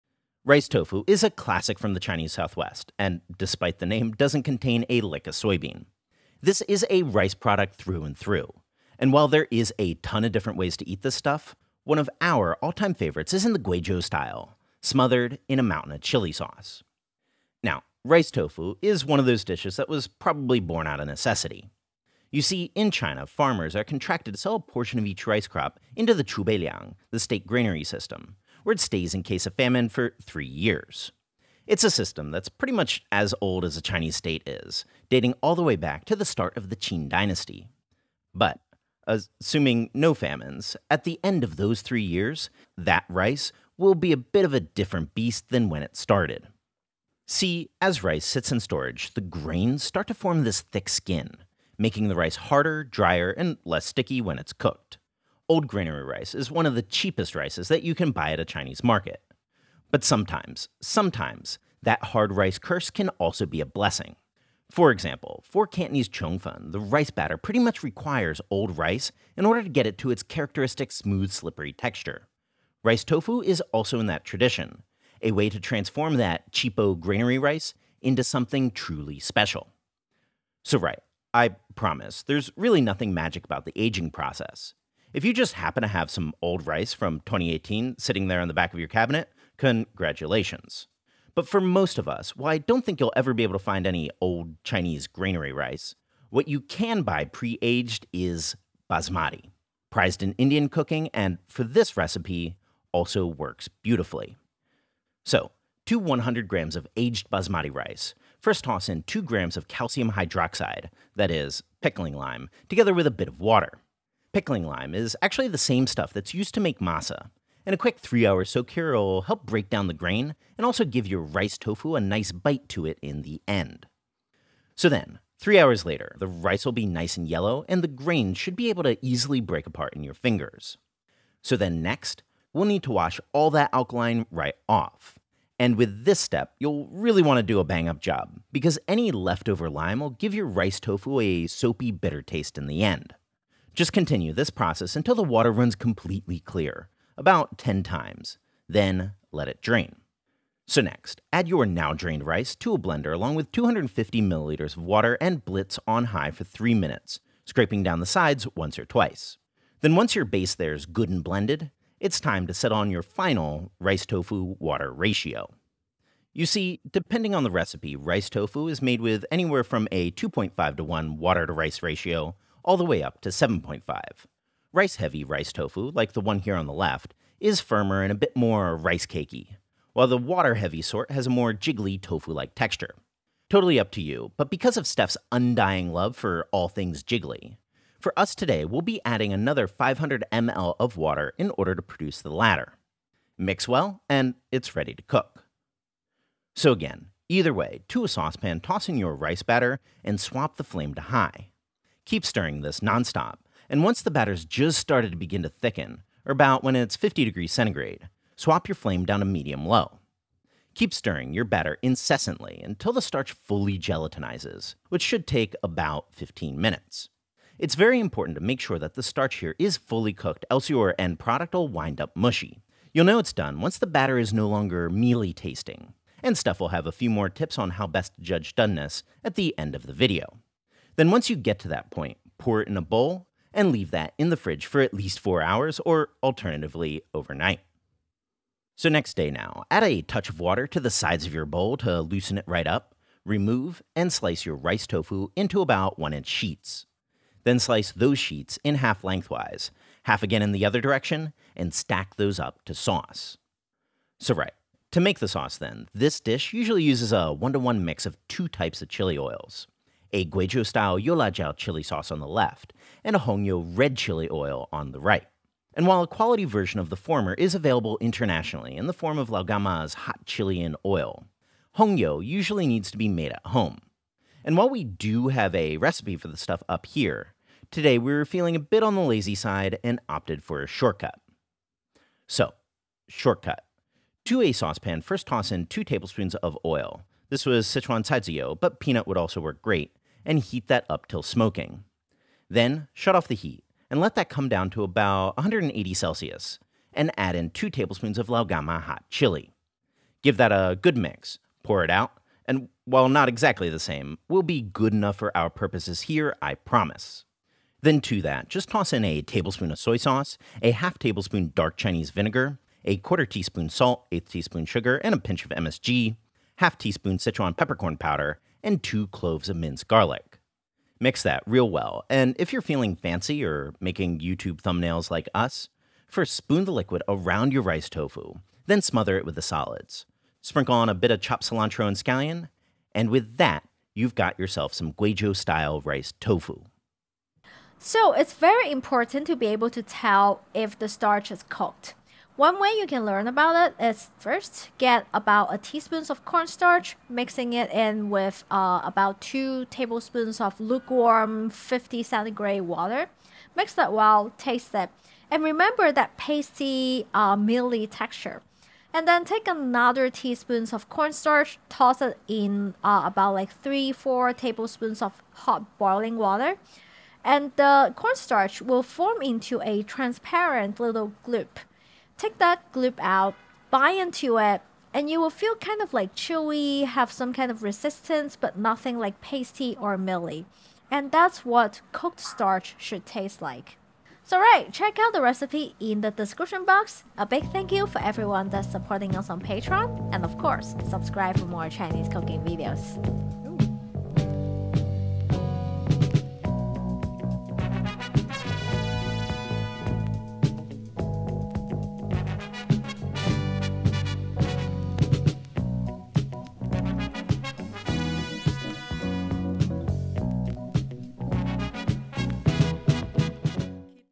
Rice Tofu, with Chili Sauce (凉拌米豆腐)
Rice Tofu! Not a lick of soybean here, this 'tofu' could be thought of as more of a... firm, slippery rice cake. Great ingredient.

0:00 - Introduction and Background
1:44 - Making Rice Tofu
4:14 - Making Chili Oil topping
5:37 - Outro & Tips for Doneness

Written recipe is also over here on /r/CasualChina, if you prefer your written recipes in Reddit form:

And there's a full, 19 minutes of uncut footage of Steph doing the 2.5:1 water to rice rice tofu here if you like:

INGREDIENTS, RICE TOFU

* Aged Basmati Rice, 100g. Or, if you happen to be China based you can find the granary rice as 陈米, or in Guangdong as 肠粉米

* Calcium Hydroxide (熟石灰), 2g.

* Water, 250mL-750mL. Water quantity depends on your desired ratio. Use 250mL for the firmer sort, 750mL for jiggly.

Note that if going for the jiggly one, we like to first blend it with 250mL of water, then add an additional 500mL after it's well blended. Less messy that way.

Also, feel free to scale up. If you're going for the firm sort of rice tofu, I'd recommend doubling the recipe.

PROCESS, RICE TOFU

1. Mix together the rice, the calcium hydroxide, and enough water to come up about ~1cm above the rice. Let it soak for ~3 hours.

2. Rinse the rice ~10 times, or until the water runs completely clear.

3. Blend together with 250mL of water, scraping once or twice.

4. If going for the jiggly rice tofu, add the extra water now and mix well.

5. Cook the rice tofu. First heat it up until it's beginning to clump. ~53C, then swap the flame to medium low. Continue to cook for 10-15 minutes, stirring constantly, until the starch tastes cooked (a bit like a boba), and the rice batter develops a sort of sheen.

6.  Pour into a bowl, toss in the fridge for at least four hours, or overnight.

INGREDIENTS, RICE TOFU WITH CHILI OIL TOPPING

* Rice tofu; one batch of the above recipe if using the firm sort/half batch if using the jiggly sort.

* Chili oil(s): 2 tbsp Lao Gan Ma Fried Chili in Oil (老干妈油辣椒), 2 tbsp homemade red chili oil -or- shortcut of 2 tbsp Lao Gan Ma Fried chili in oil cooked with 2 tbsp of peanut oil/caiziyou. This is the Lao Gan Ma we're talking about You can also totally sub in the Lao Gan Ma chili crisp if that's what's in your cupboard.

* Light soy sauce (生抽), 1 tbsp.

* Dark Chinese vinegar (陈醋), 1/2 tbsp. Or you can try to sub with a 2:1:1 ratio of balsamic to white vinegar to water

* Seasoning: 1/4 tsp salt, 1/8 tsp sugar, ~1/16 tsp MSG (味精)

* Sichuan peppercorn powder (花椒粉), 1/2 tsp.

* 2 cloves worth minced garlic

* Cilantro and Scallion, for garnish.

PROCESS, RICE TOFU WITH CHILI OIL TOPPING

1. If using our shortcut - heat up 2 tbsp of the caiziyou or peanut oil until smoking, then shut off the heat. Wait til the oil's down to ~180C, ~90 seconds later, then add in the Lao Gan Ma and mix.

2.  Mix together all the ingredients, smother over the rice tofu. Top with chopped cilantro and sliced scallions.

Footage of the rice harvesting is courtesy of the channel "Xin Ge Vision". Was very happy to find some actual, modern day rice harvesting in China rather than the cliche 'grasping rice' that you usually find in stock footage. Link to the full video is here (in Chinese):